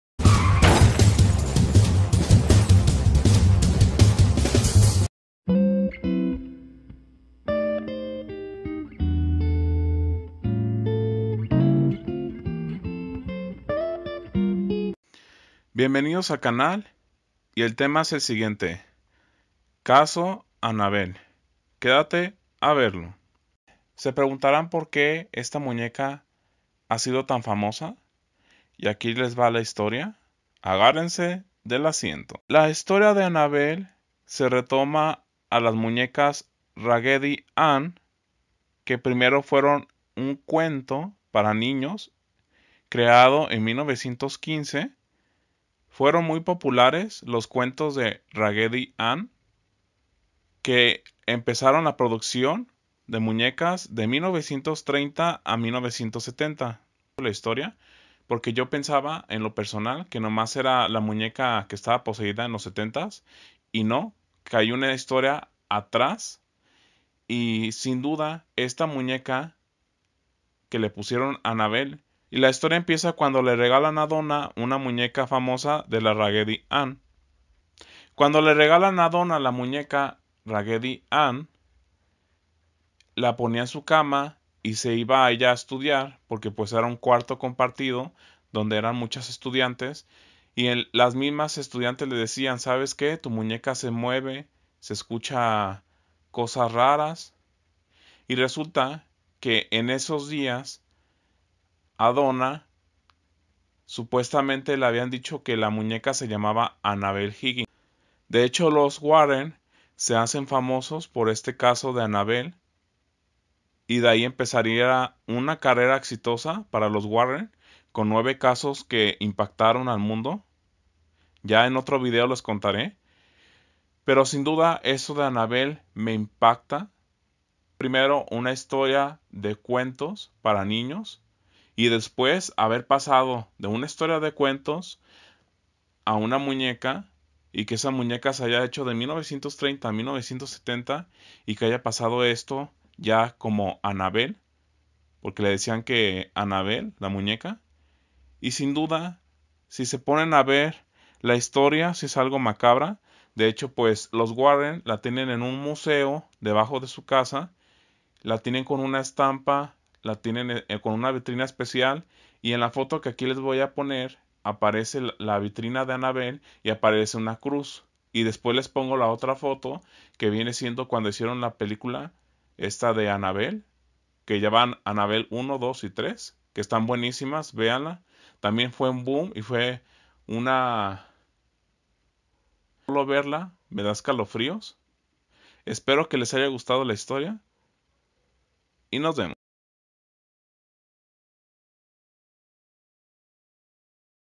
Annabelle muñeca diabolica caso
Annabelle muñeca diabolica caso quedate a verlo,el famoso caso de la muñeca poseida en los 70